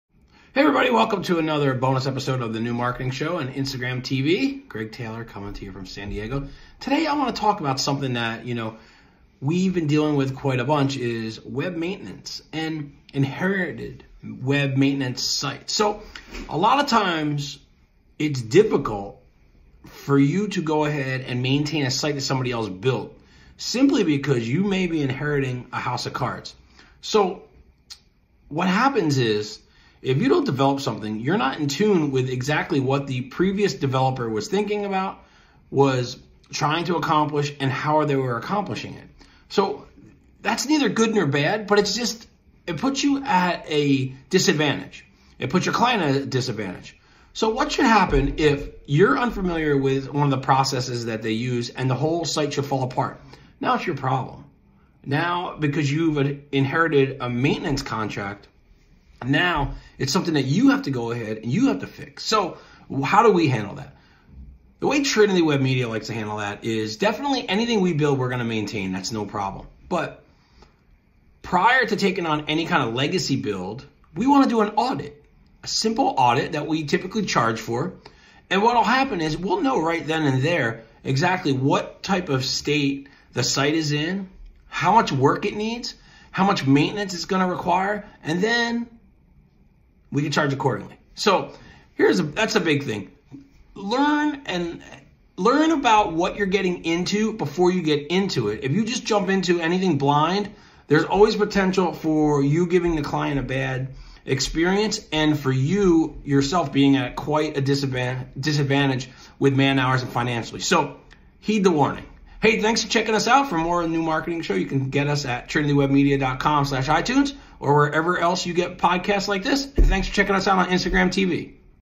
Inheriting Website Maintenance
It's often difficult to perform maintenance on websites that your team didn't develop.

More often then not, you'll find yourself inheriting a house of cards.